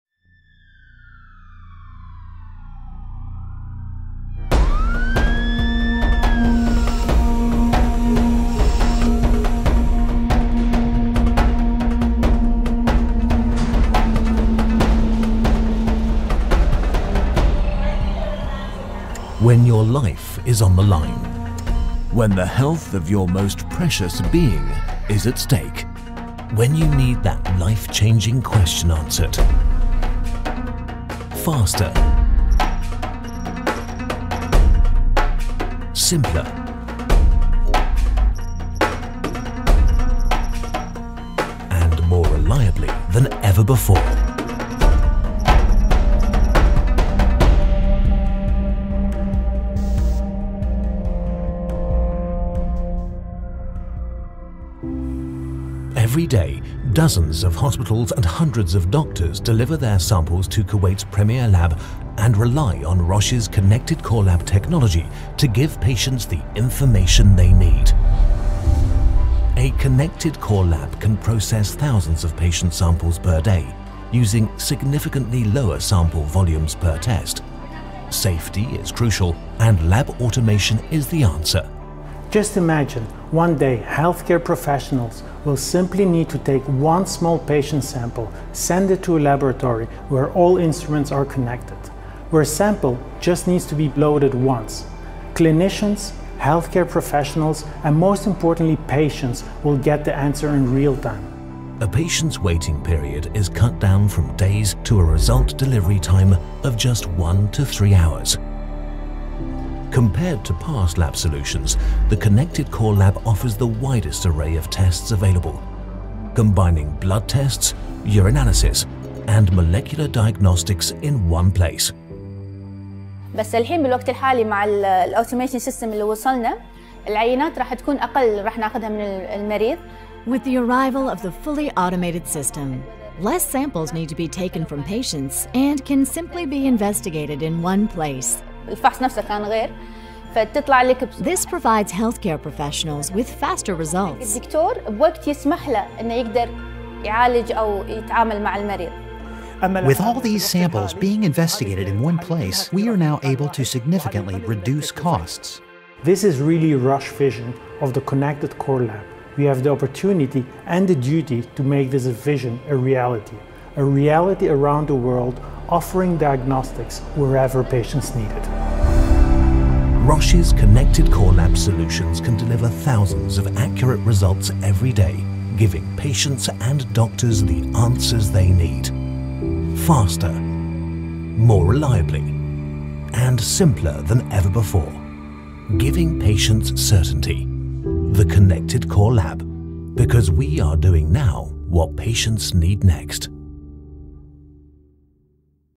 Realising the integrated lab vision in Kuwait
This is Roche's vision of the Integrated Core Lab. Imagine - every day, dozens of hospitals and hundreds of doctors deliver their samples to Kuwait's premier lab and rely on Roche's Integrated Core Lab technology to give patients the information they need.